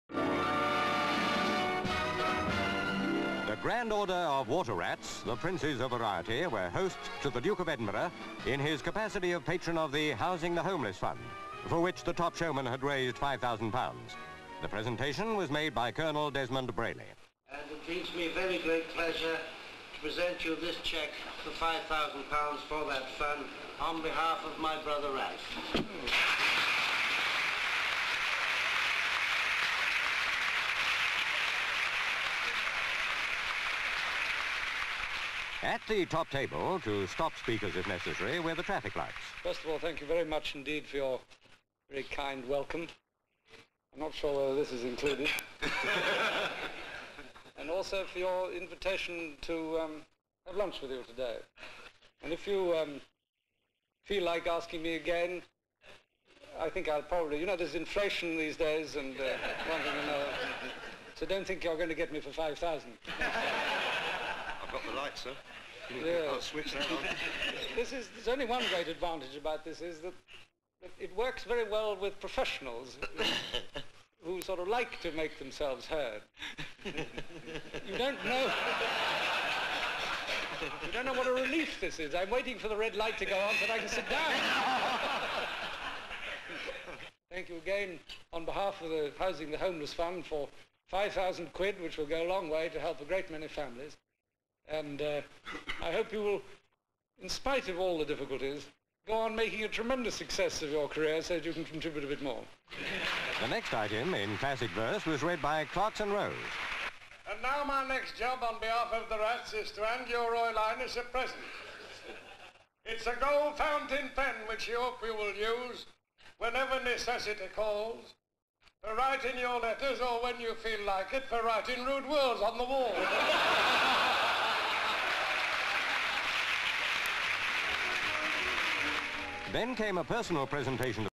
Water Rats Fete Duke (1965)
Title reads: "WATER RATS FETE DUKE".

Europa Hotel, London.

L/S of charity luncheon held by the Grand Order of Water Rats.  C/U Prince Philip (Duke of Edinburgh) and King Rat / comedian Tommy Trinder.  M/S members of the Water Rats. M/S comedian Ken Dodd pulling faces.  M/S cheque being presented to Duke (natural sound).  M/S members clapping. Various shots of Duke's speech - he cracks lots of jokes.  M/S members clapping.  M/S Clarkson Rose, Deputy Preceptor, reading comic monologue.  M/S members applauding.  L/S Clarkson Rose presenting Duke with gold fountain pen. M/S ditto.

Cheque is for Â£5000 and will be presented to the Housing the Homeless fund.

 FILM ID:1820.25